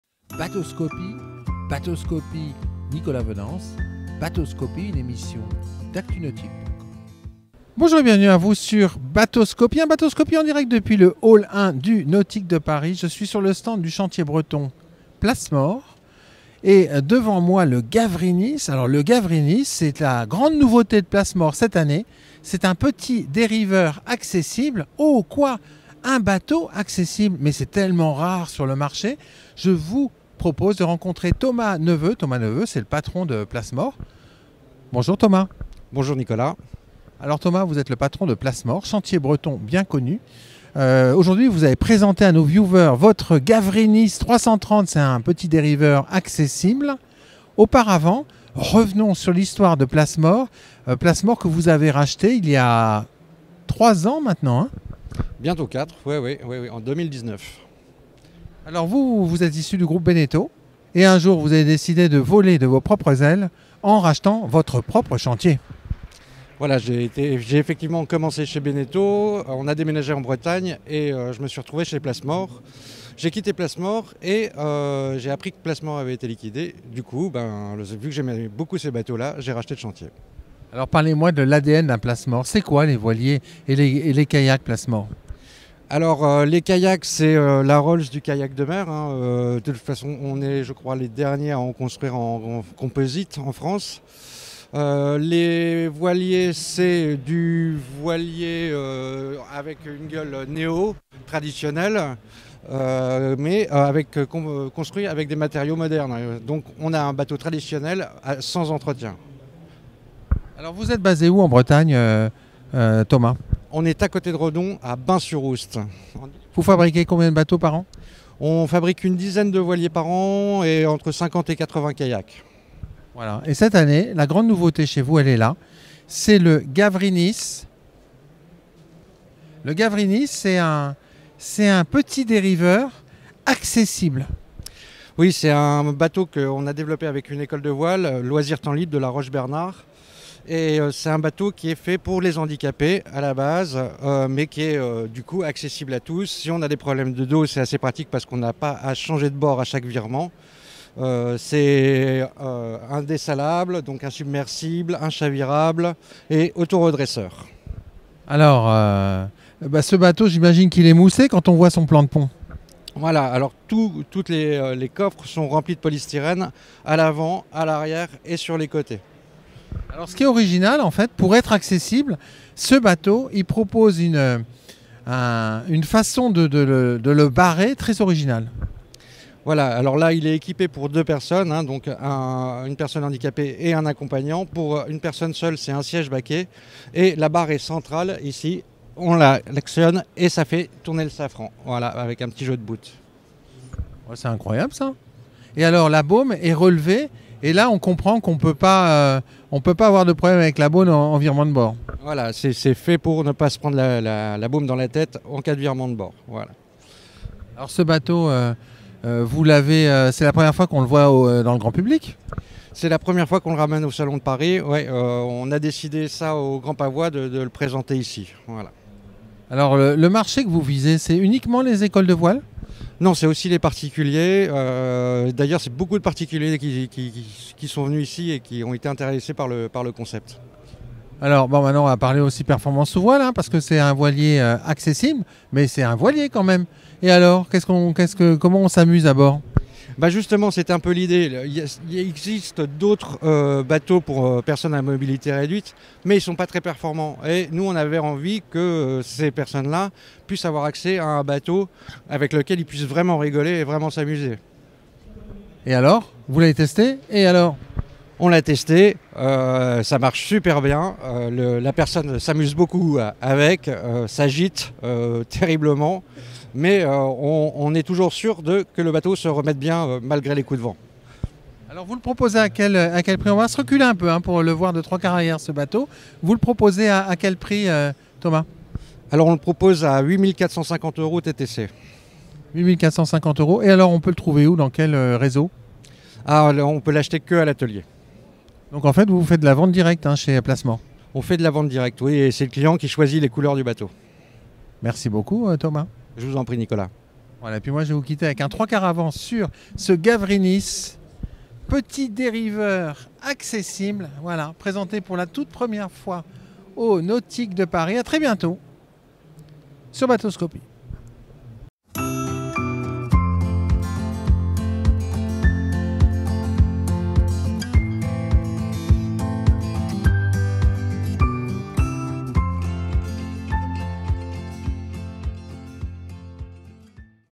Nautic Paris 2022 - Gavrinis, le petit voilier accessible aux PMR, du breton Plasmor
Sur le Nautic de Paris 2022, qui avait lieu Porte de Versailles, comme tous les ans, le phantier breton Plasmor, présentait un bateau dont le concept est bien trop rare sur le marché : un petit dériveur accessible, dont l'objectif est de permettre aux personnes à mobilité réduite, de faire de la voile et d'y prendre plaisir !
Le Gavrinis, tel est son nom, est insubmersible et organise son cokpit de telle façon qu'une personne handicapée, puisse manoeuvrer son voilier toute seule.
Génial !!